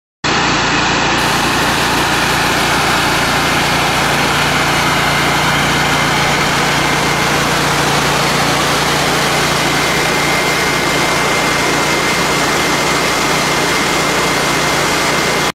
FM120 Perfectly balanced ATV flail mower - Glass of Wine Test!
ATV Flail Mower short video sent to us by a satisfied customer showing a full glass of wine perfectly balanced on the Chapman ATV flail mower, with the motor running, testing the smoothness of the machine. The rotors of this FM120 ATV flail mower are dynamically balanced to "G6.3" standards in-house whereas other manufacturers are typically 18 to 22 balanced!
Combine this with the fantastic Honda 21HP V-Twin 630cc engine with a built-in oil pump, low oil alert system for added engine protection, and highest specification, and you get the best ATV flail mower for paddock and field maintenance.

E&OE - Supplied with the manufacturer's warranty.